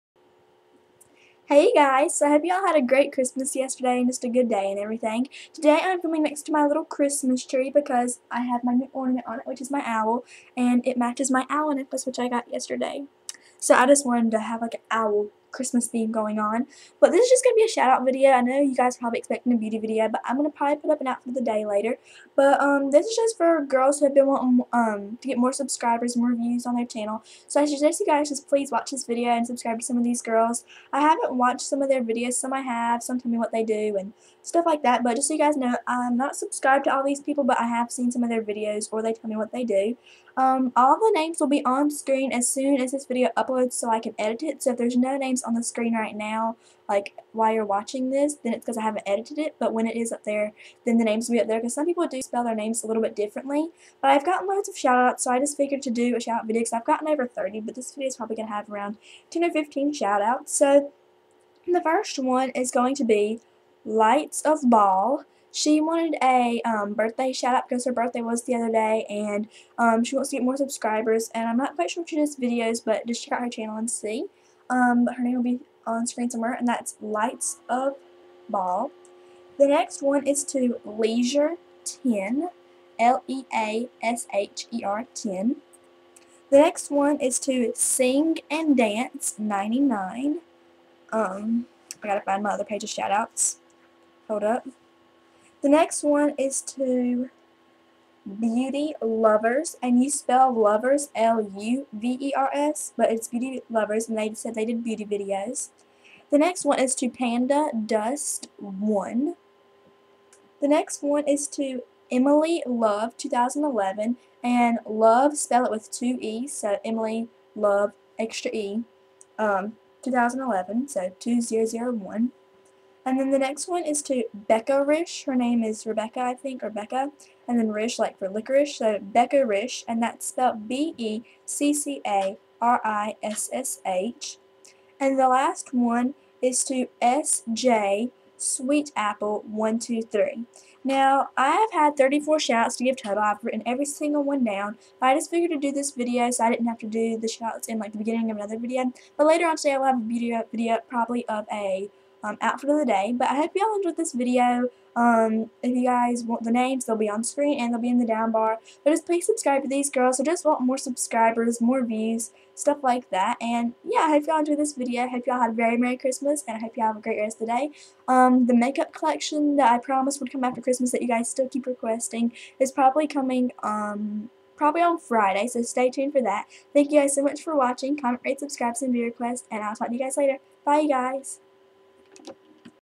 Shoutouts!
Names will be down here soon! This is just a video for some more shoutouts! I decided to do a shoutout video instead of putting them in will another video so i hope you all enjoyed! If any of yall want a shoutout for the month of December let me know!